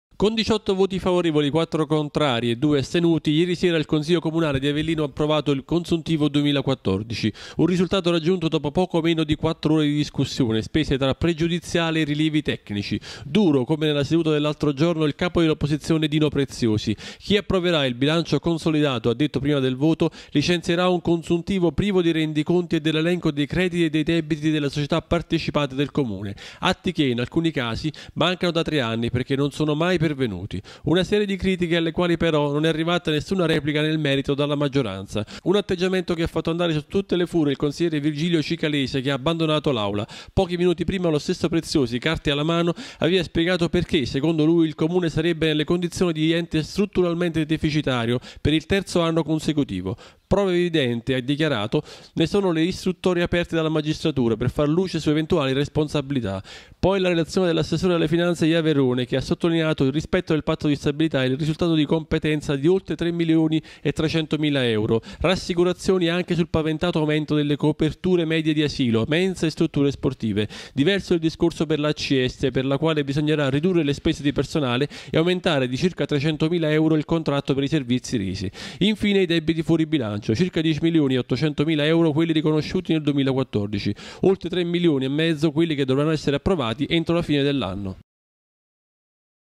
Avellino, ok al bilancio consuntivo in aula
Via libera dal consiglio comunale al consuntivo. Il bilancio passa con i soli voti della maggioranza tra le proteste della minoranza. Preziosi attacca: non c'erano i presupposti per approvarlo.